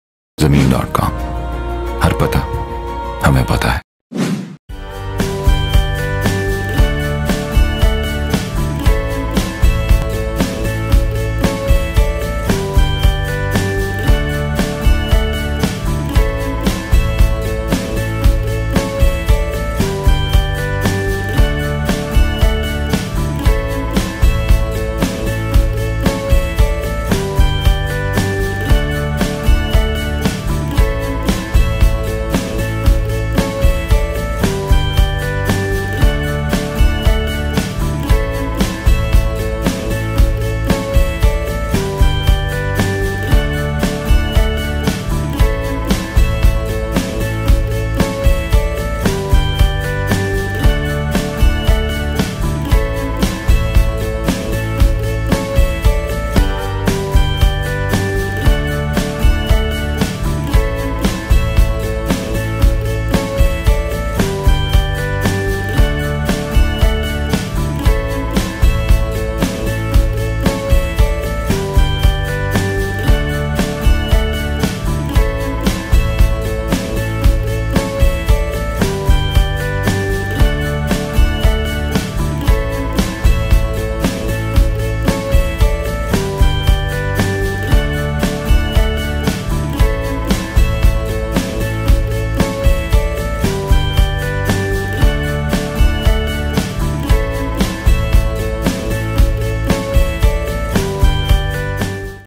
4 MARLA OFFICE FOR RENT IN BLOCK DD PHASE 4 DHA LAHORE
Here is what you have been looking for!
We have this hall having an area of 4 Marla available for rent in DHA Phase 4, Lahore.
Ideal Location.

Features:
1) Hall
2) Room
3) Kitchen
4) Bathroom
5) Terrace

We are offering it for only Rs 48,000/-.

Do not forget to mention Zameen. com when calling